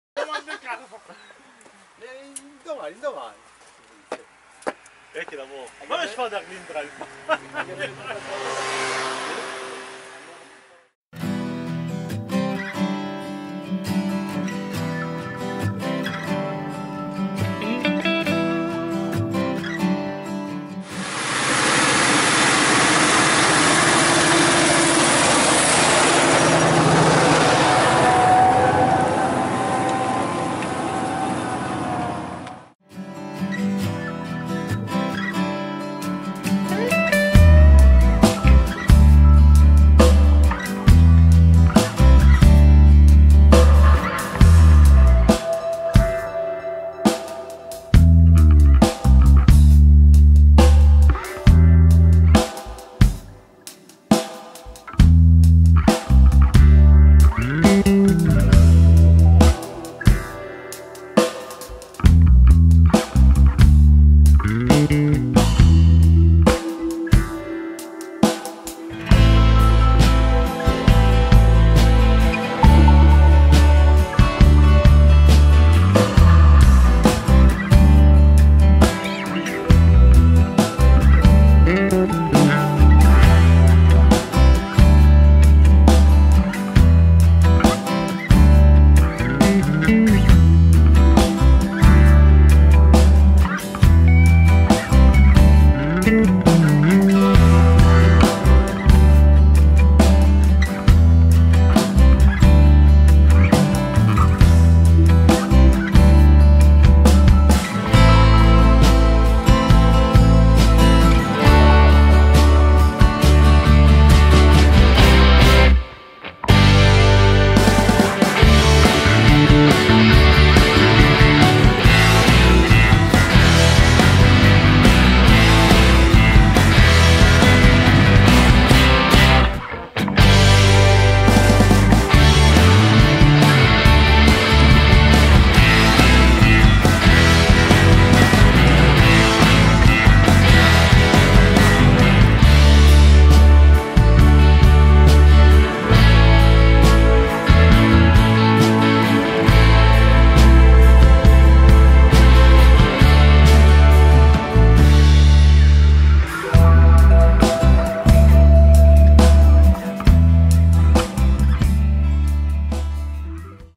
Fendt 942 Vario
Top di gamma. Due trasmissioni, due impianti idraulici indipendenti. Un solo motore, ma d’eccezione. Con il Man da 9 litri, Fendt torna all’antico, pur essendo al tempo stesso incredibilmente moderna. Il nostro test con quadrivomere su terreno bagnato